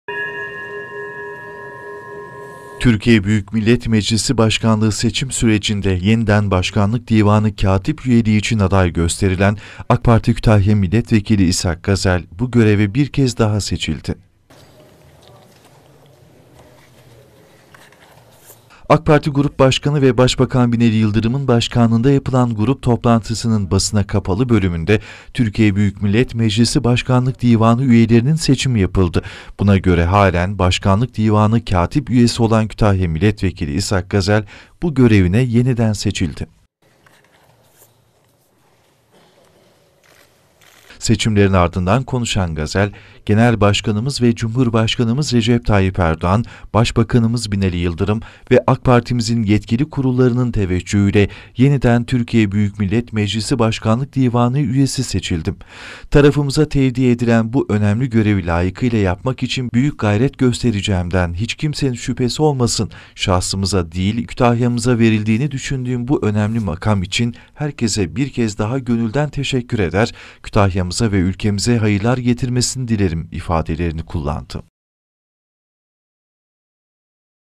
MİLLETVEKİLİ GAZEL YENİDEN BAŞKANLIK DİVANI KATİP ÜYESİ OLDU
Haber Metni:

MECLİS BAŞKANLIĞI SEÇİM SÜRECİNDE BAŞKANLIK DİVANI KATİP ÜYELERİNİN SEÇİMİ YAPILDI. DAHA ÖNCE BU GÖREVİ YÜRÜTEN AK PARTİ KÜTAHYA MİLLETVEKİLİ İSHAK GAZEL BİR KEZ DAHA BAŞKANLIK DİVANI KATİP ÜYESİ OLDU.